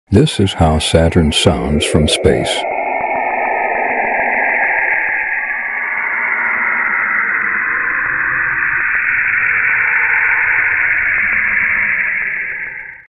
The Creepy Voice Of SATURN 🪐😱🚀 Space Sounds part.6
Journey to the heart of our Solar System with the true sounds of Saturn. This audio odyssey uncovers the hidden melodies of the ringed planet. From the ethereal chimes of its vast rings to the deep bass notes of its swirling storms, Saturn reveals a universe of sound waiting to be explored.

Saturn, known for its breathtaking beauty and stunning complexity, also sings a song few have heard. Leveraging state-of-the-art space recording tools, we bring Saturn's symphony to your ears. It's a soundscape that marries science and sensation, astronomy and art.

Ready for a cosmic concert? Plug in, relax, and drift away with Saturn's unique serenades.

💬 Share your thoughts on Saturn's soundscape in the comments section.
🪐 Elevate others' playlists by sharing these epic Saturn sounds.